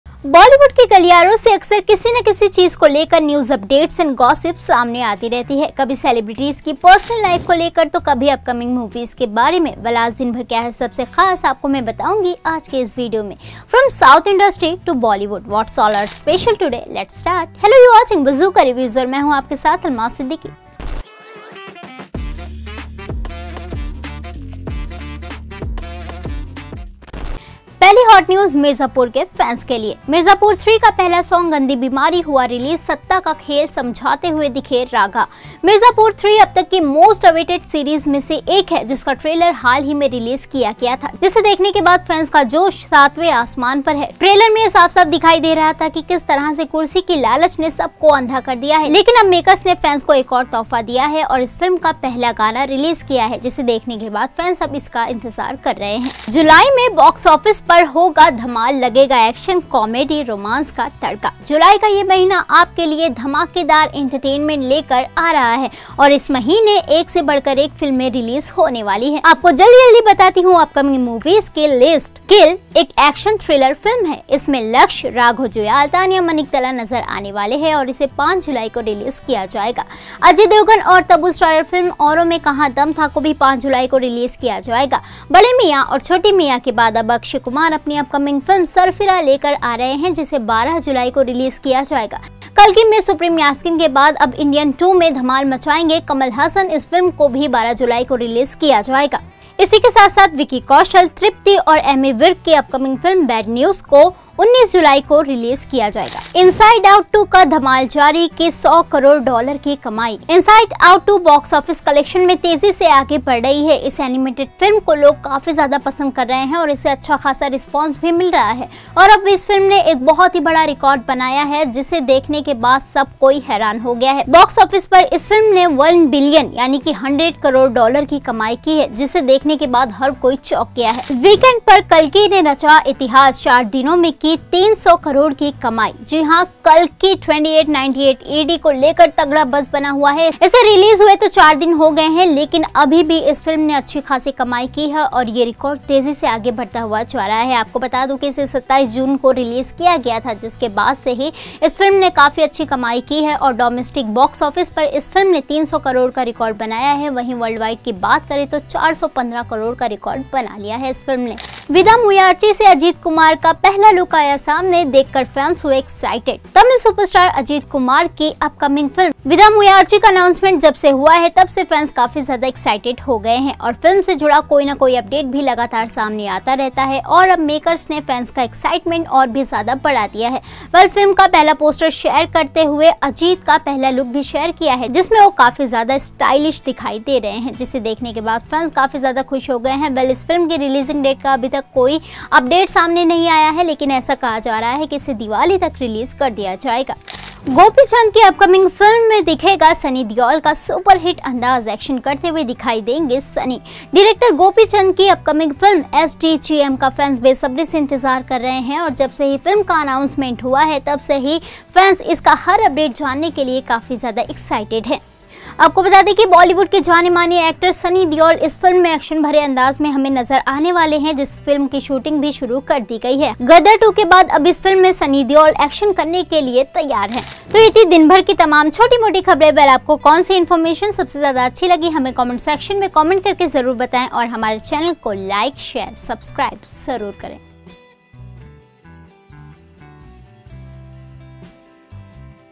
July में Box Office पर मचेगा बवाल, Release होंगी एक से बढ़ कर एक फिल्में
Discription